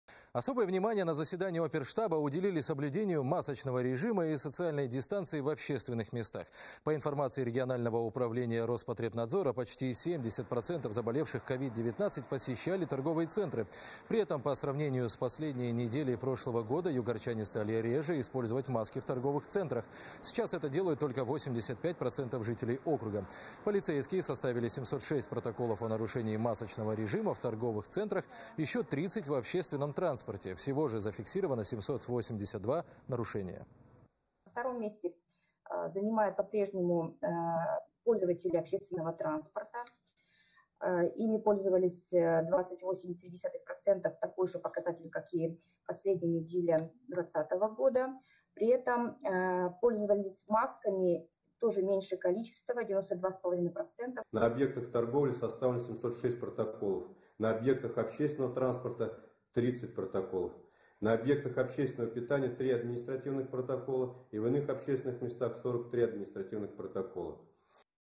Более 700 нарушений масочного режима за неделю зарегистрировали в магазинах Югры
Особое внимание на оперштабе уделили соблюдению масочного режима и социальной дистанции в общественных местах.